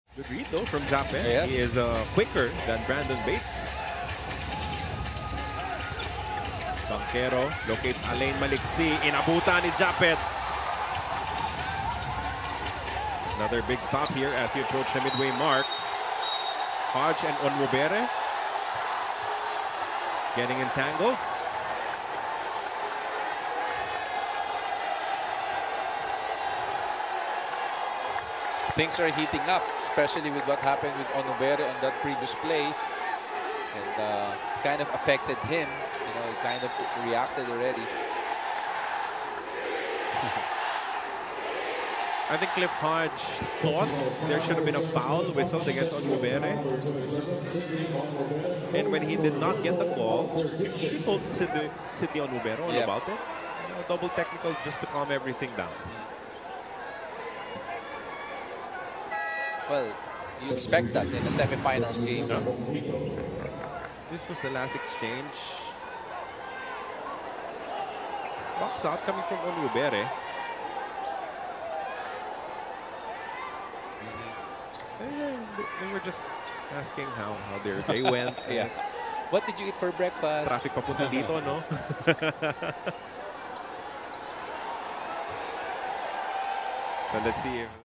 Onwubere, Hodge GETS HEATED in Game 1 of Brgy. Ginebra vs Meralco 👀 | PBA SEASON 48 PHILIPPINE CUP
PLAYOFF ATMOSPHERE 😤

Double technical for Brgy. Ginebra’s Sidney Onwubere and Meralco’s Cliff Hodge after this face-to-face altercation at halfcourt!